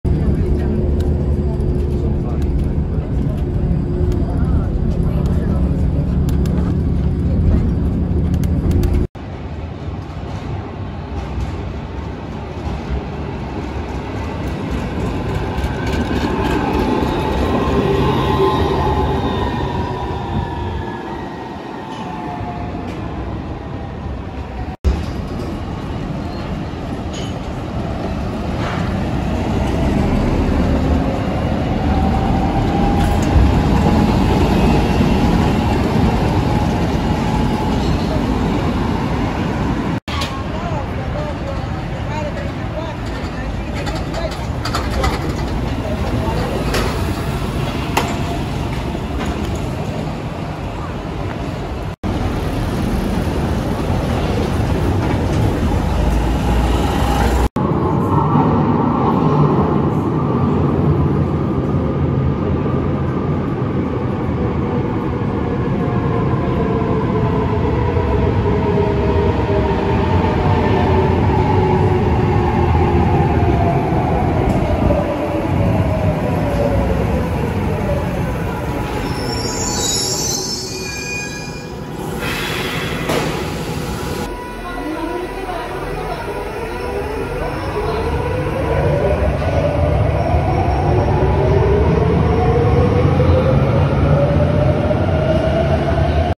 Public Transport in NAPLES, Italy | Tram, Trolleybus, Metro | July 2022
Please enjoy my short 4K UHD overview video about electric zero-emission public transport in Naples, Italy. There are few footage captures with trams, trolleybuses and metro. Captured in central part of the city in July 2022.